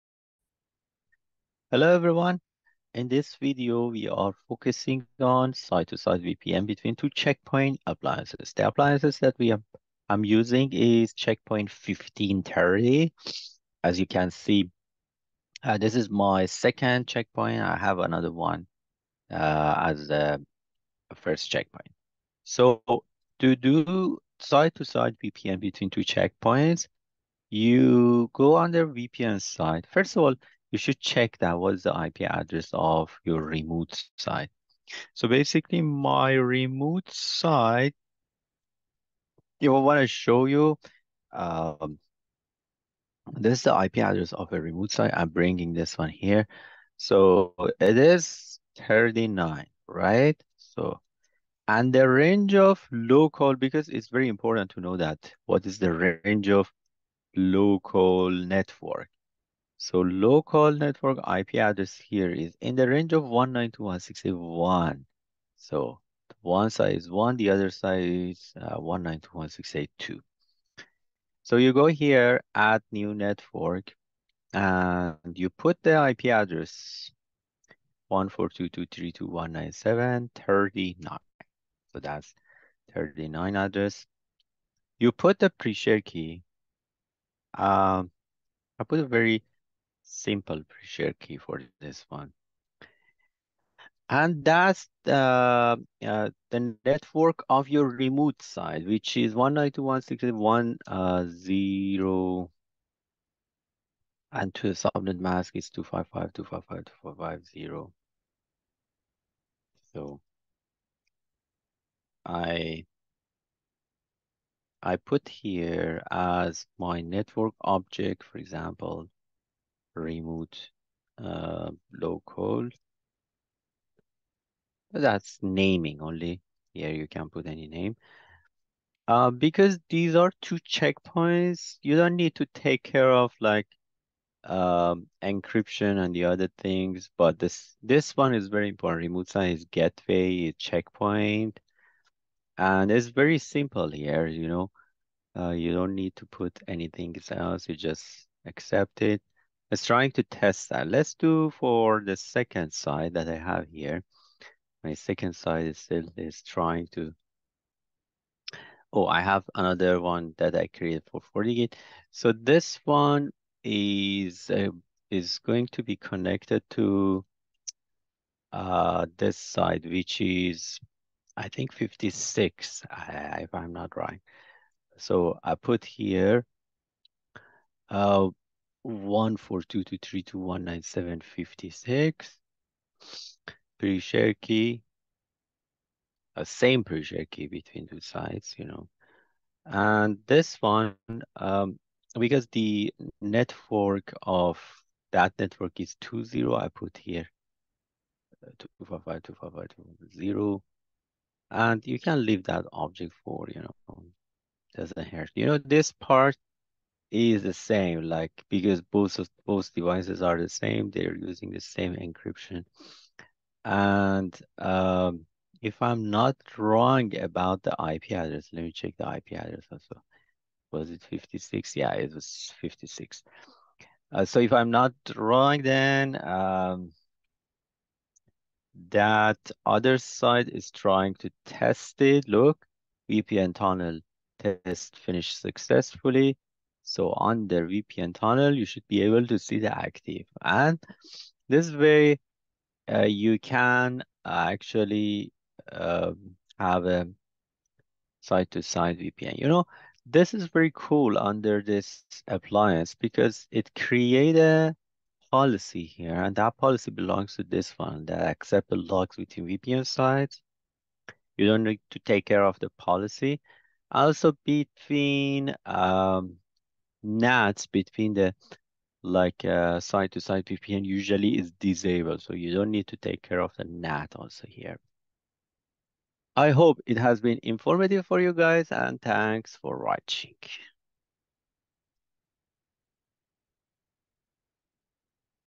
site to site VPN(checkpoint to checkpoint)
How to create a site to site VPN between two Checkpoint appliances